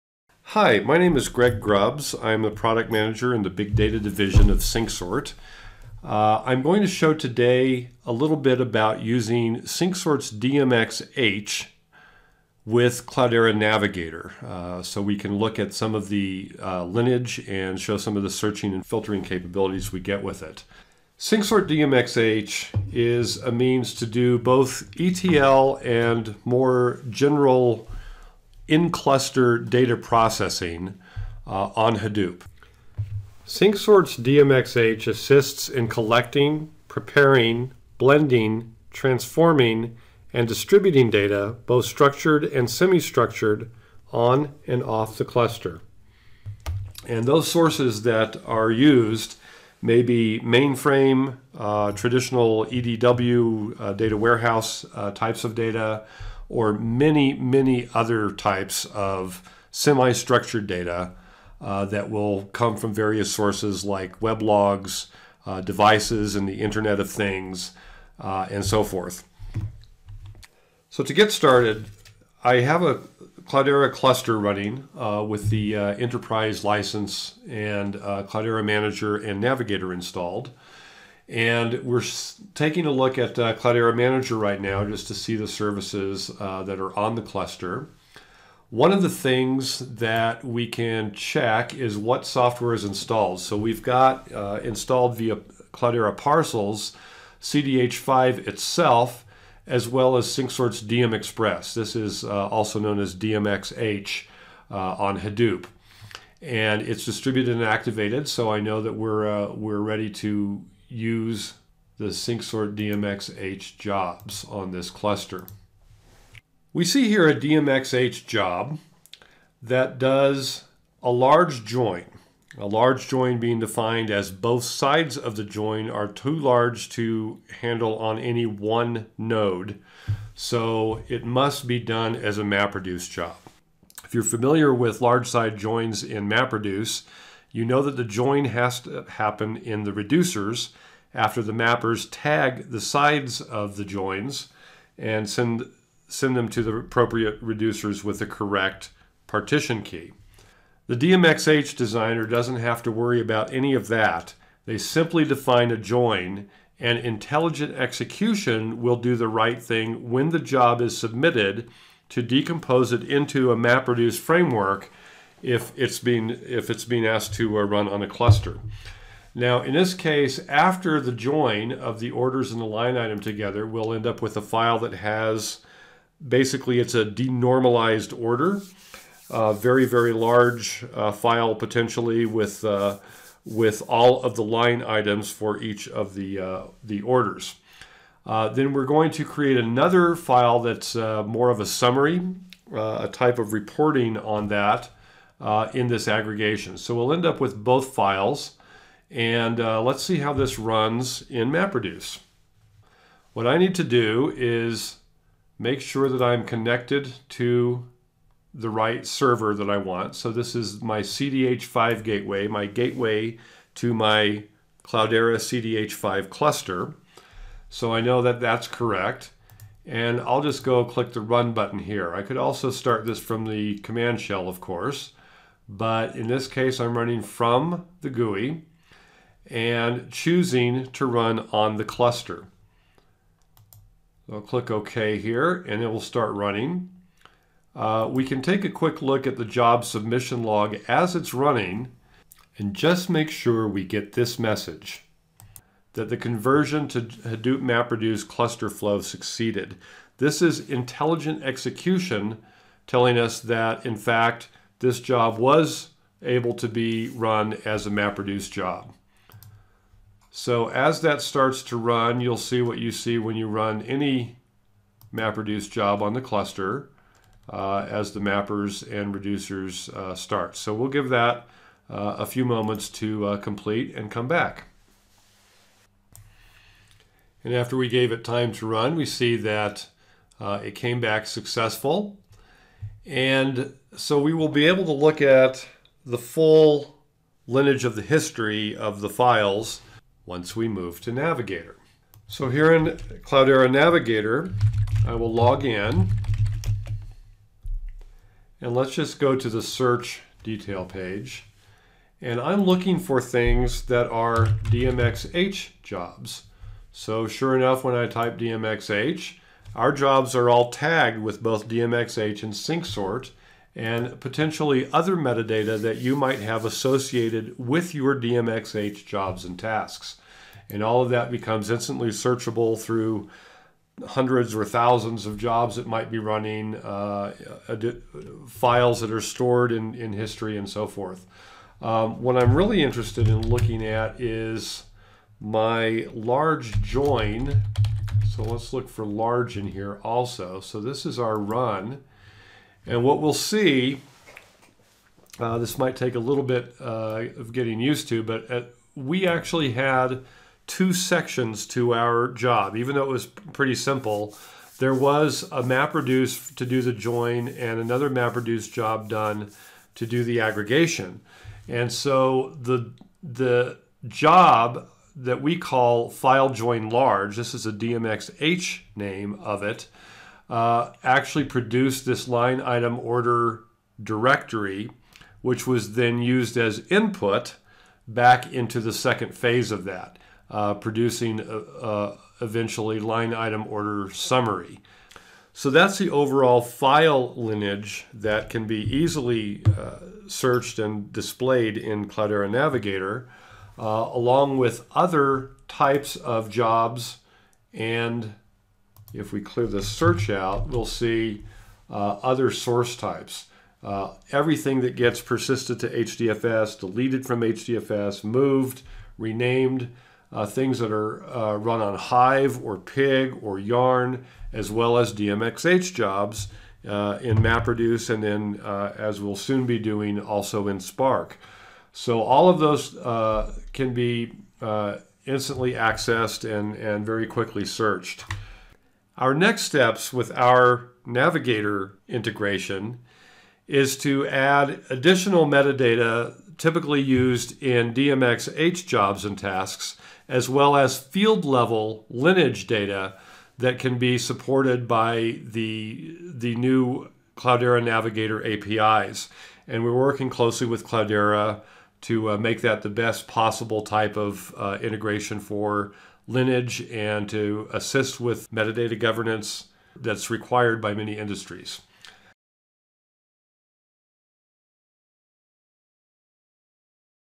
Syncsort DMX-h and Cloudera Navigator
Use of Cloudera Navigator to find and display metadata from DMX-h jobs.

Table of Contents:

00:00 - Introduction
00:20 - DMX-h Overview
01:09 - Cloudera Manager
01:56 - DMX-h Job
03:40 - Run on cluster
04:50 - Cloudera Navigator